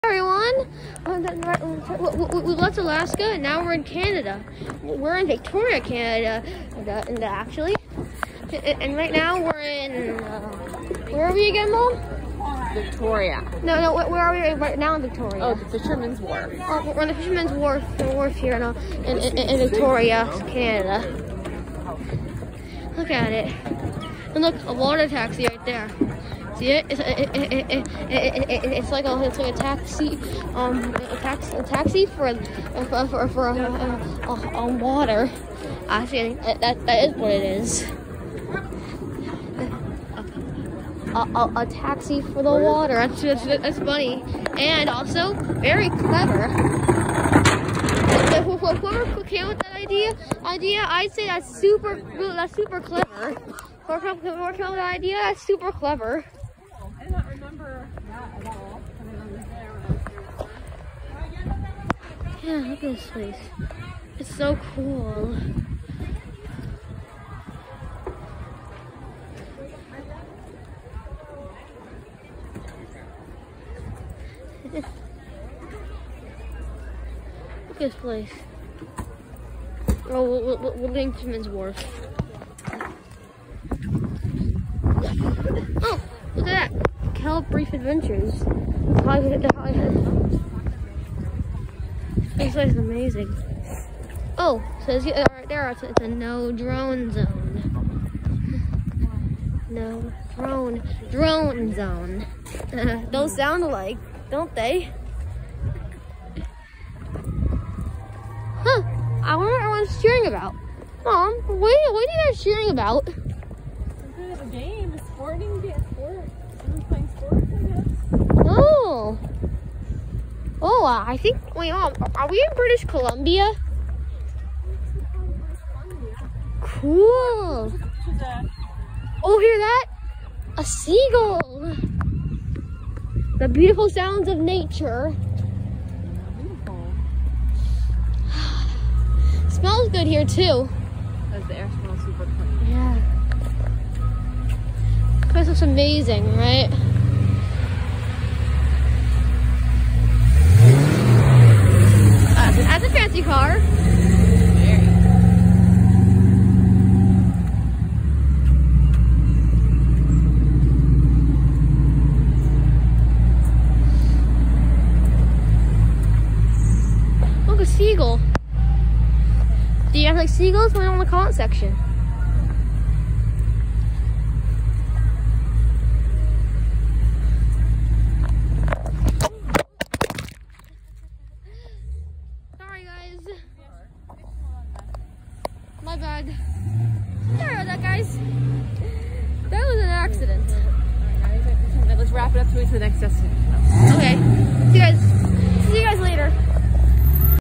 Victoria, Canada! 🇨🇦
Today, Matthew has left Alaska and is in a different country: Canada! And right now, he’s at Fishermen’s Warf!